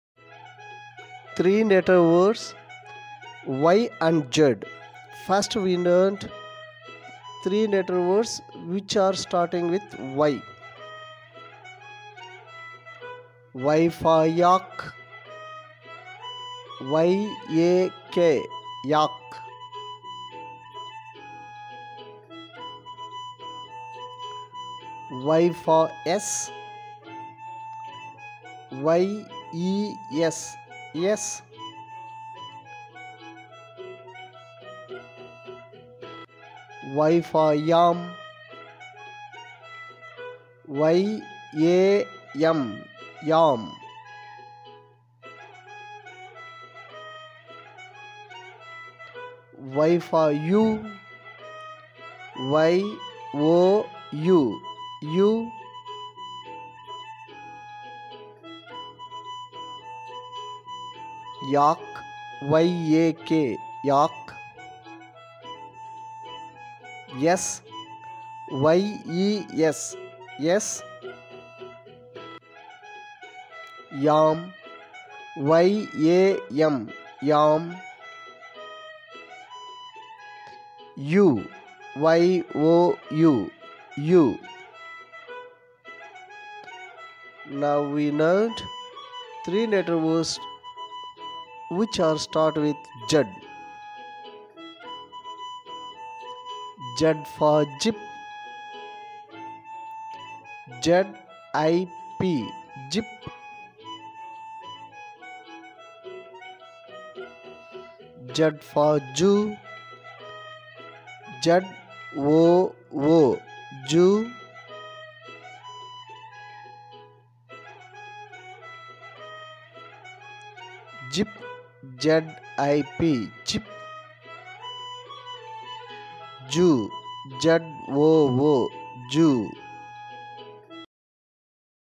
Three letter words Starts with - Y & Z
Three letter words Starts with - Y & Z. These words showing  with pictures and spelling  effectively.